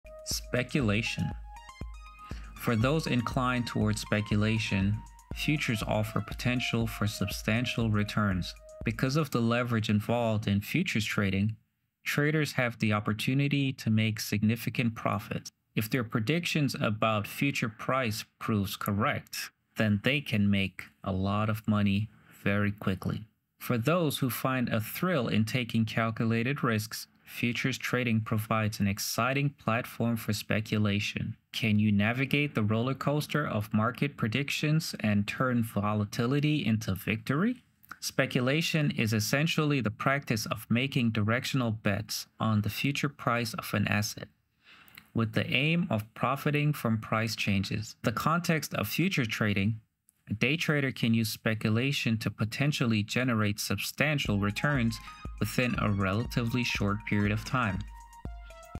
The Allure of Futures Trading Speculation
In The Allure of Futures Trading Speculation, we explain how futures contracts are used for speculative purposes. With the potential for significant profits, speculation is a key element in the allure of futures trading but comes with high risk. ftdlfctpadt"

Amazon Music